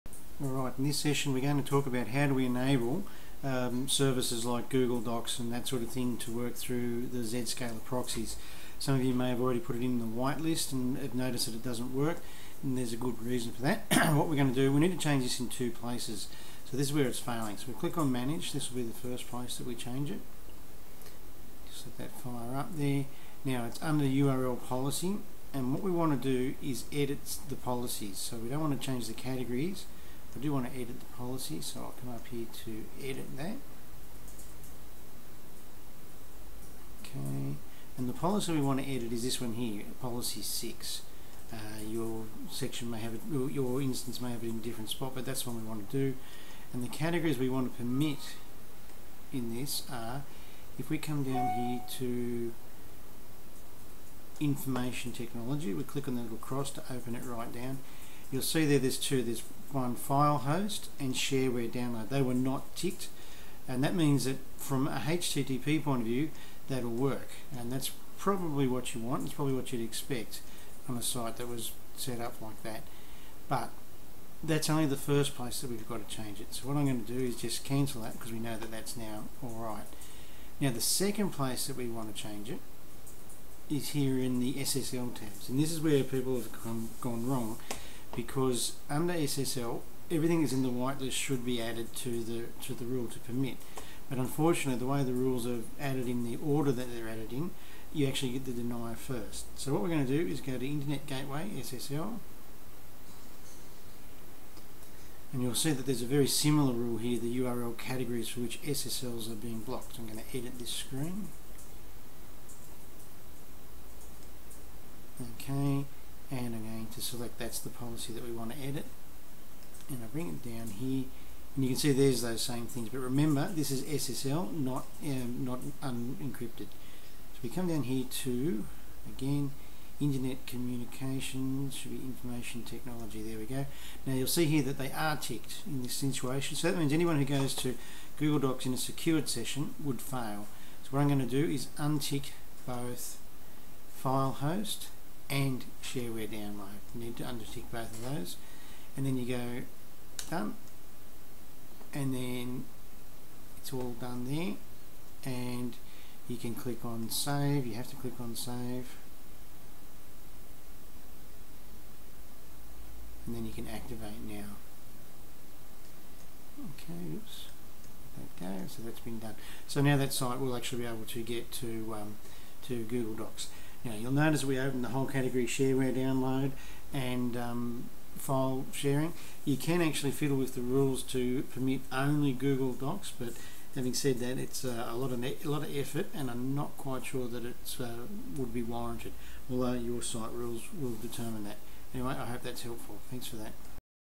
CEnet: Zscaler - 6. How to enable access for Google Docs
This video is a quick walk through on how administrators of CEnet Zscaler instances can allow access to Google Docs as the default CEnet template inherently blocks this.
This is a video compiled by Alan Ibbett - Catholic Education Office, Diocese of Wollongong